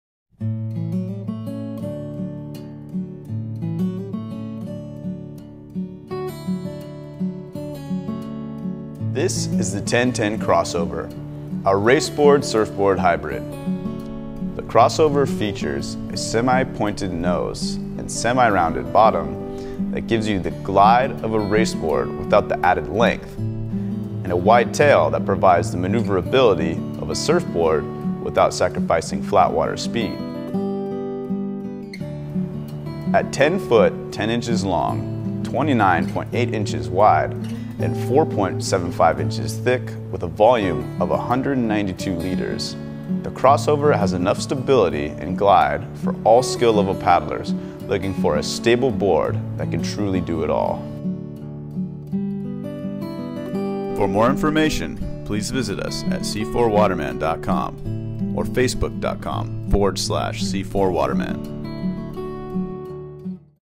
C4 Waterman 10'10" Crossover Touring Standup Paddleboard
Do you want a touring board, but don’t want the hassle of dealing with the size? The Crossover has a rounded, displacement hull so you can cut through the water with ease and a wide tail for stability and maneuverability for when you want to catch a few waves along the way. With deck inserts to add bungees, you can bring every essential your heart desires for a long day on the water, from sunrise to sunset.